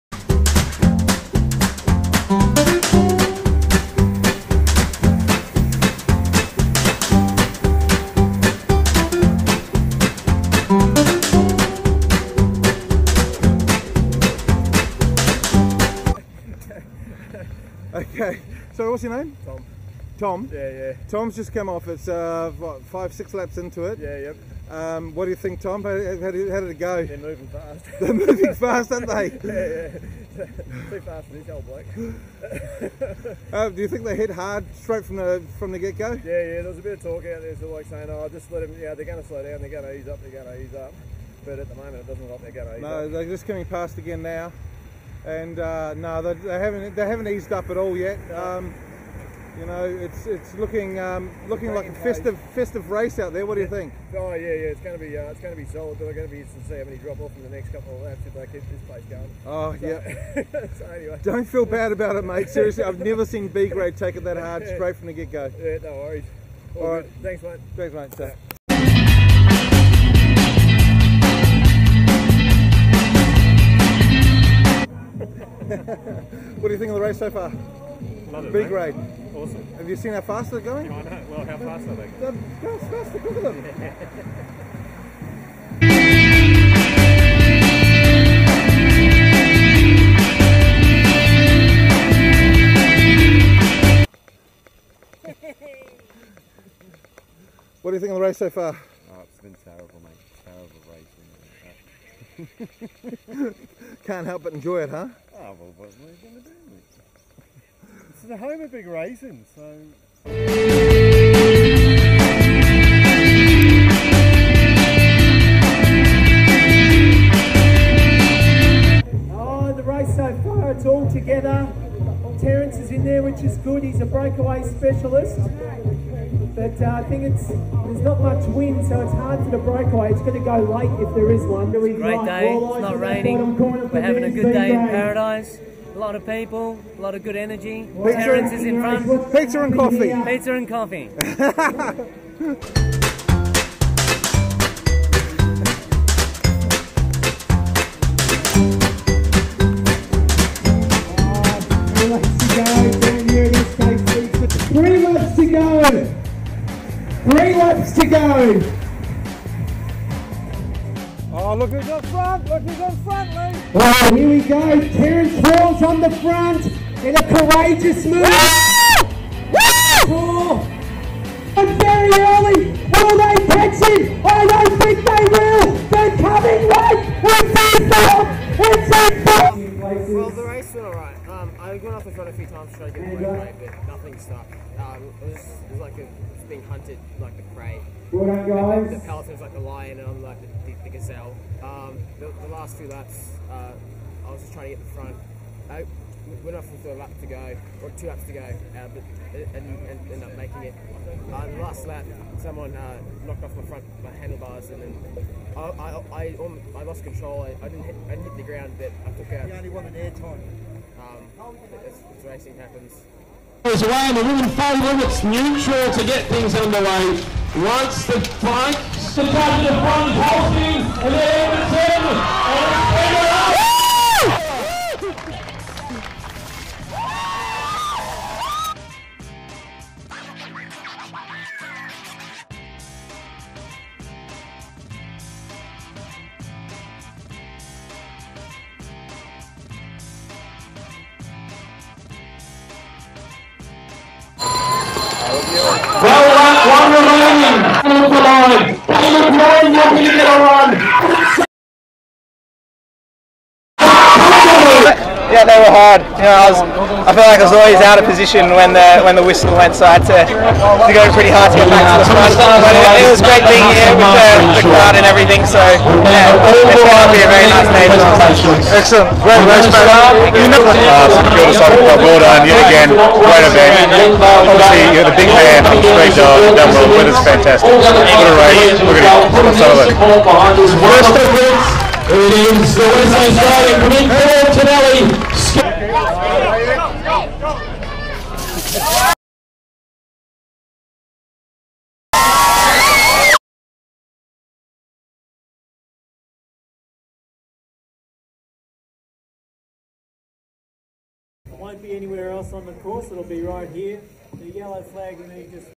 SKCC SUPERCRIT VLOG
A massive day of racing at Port Melbourne held by the ST Kilda Cycling Club. We saw the best riders in the country and the fastest race of the year.